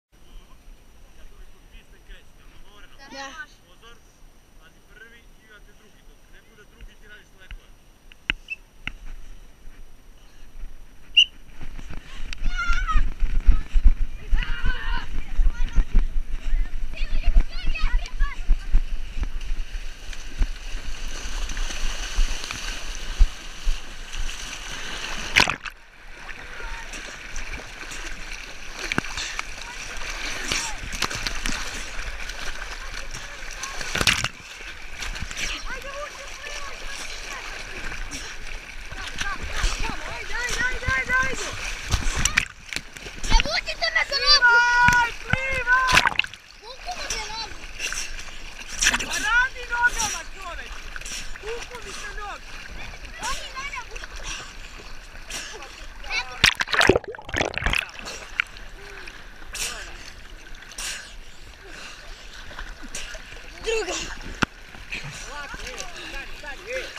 Do tramboline, Alexandra kamp - škola sporta Sporticus
Letnji sportski kamp, škole sporta Sporticus - prvi dan.
Alexandra camp, Grčka, 2014. god.